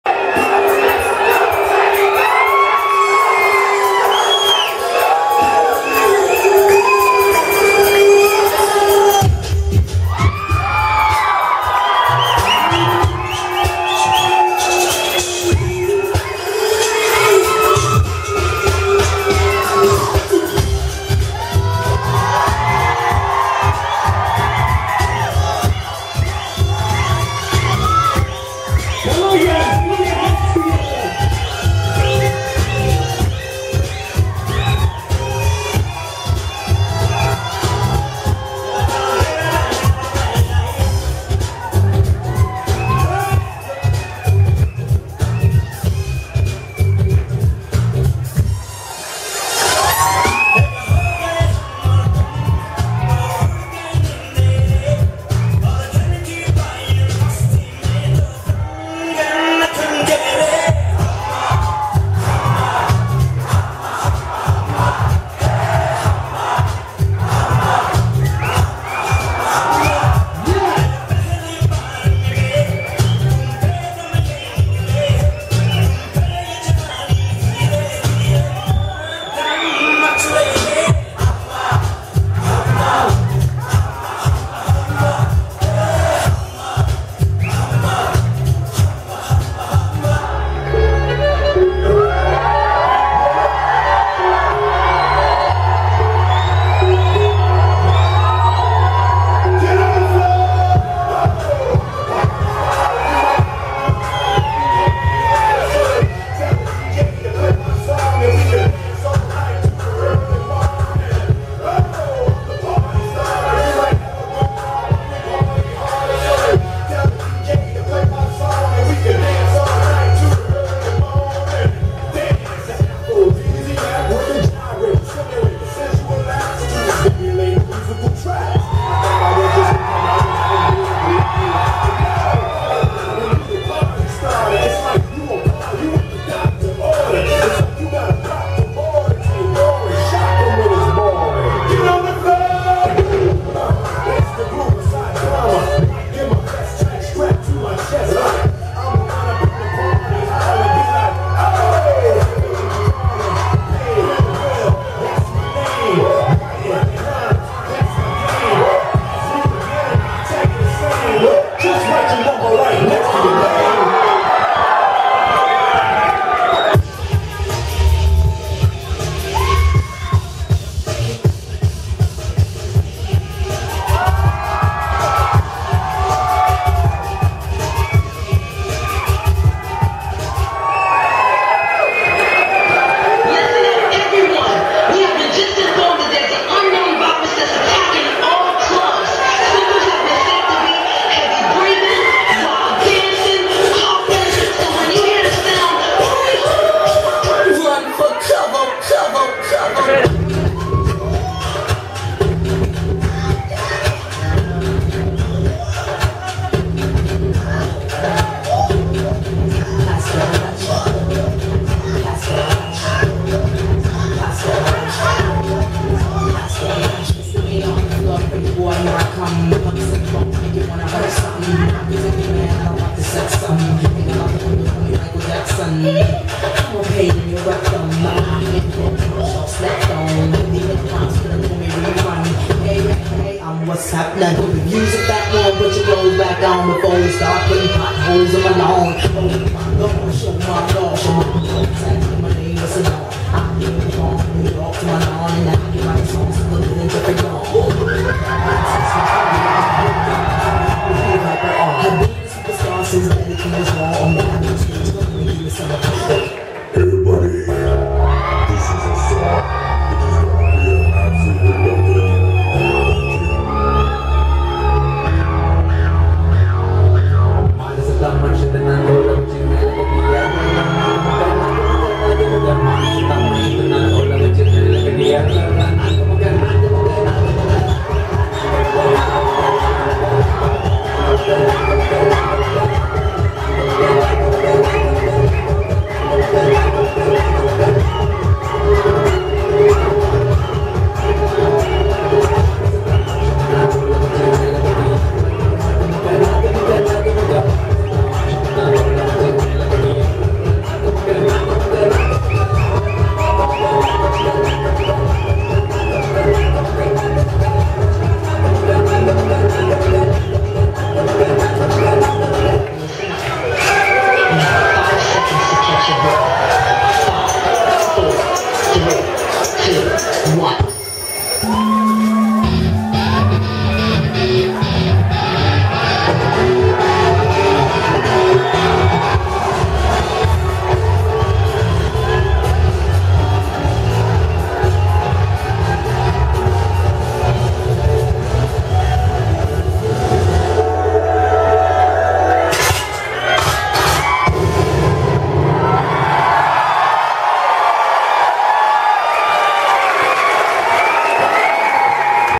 Dance Freakers Performance @ Arclights Show
Location: HCL Technologies Ltd., Bangalore.
Group Name: Dance Freakers
Members: Suresh Babu S, Srinath M, Nagaprasad Ms, Rajesh Kumar Manubolu, Sanaya Yanush Wadia, Aradhana Biswal, Nanda S, Swathi Karmakar
Choreography : Suresh Babu